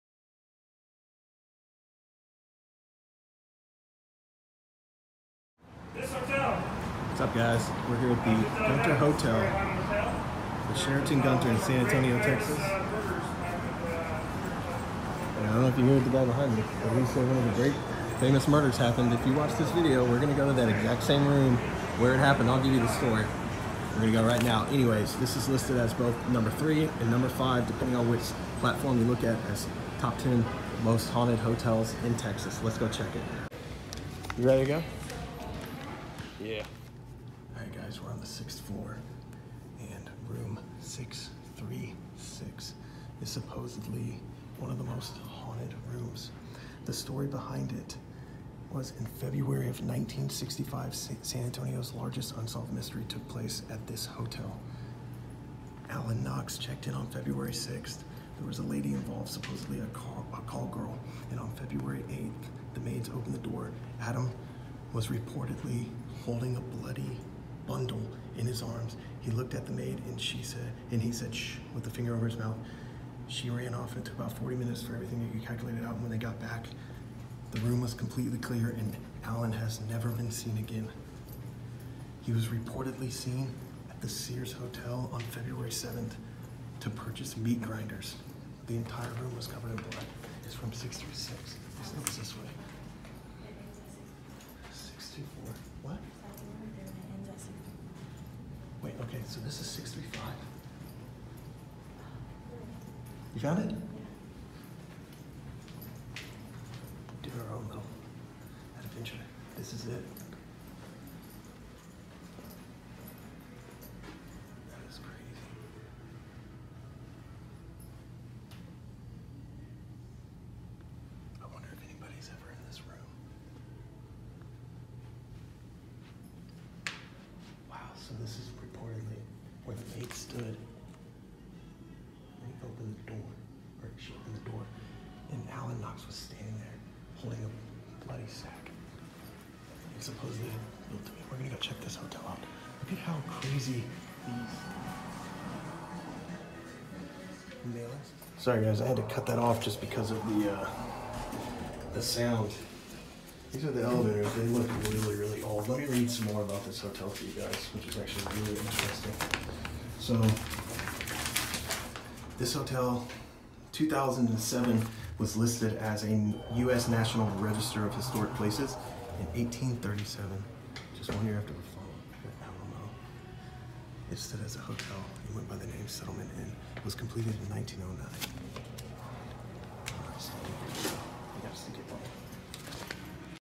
Sheraton Gunter Hotel - Standing At The Hotel Room Door of One of San Antonio’s Biggest Mysteries
Due to requests, I set up a donations avenues in the event you wanted to contribute to the travels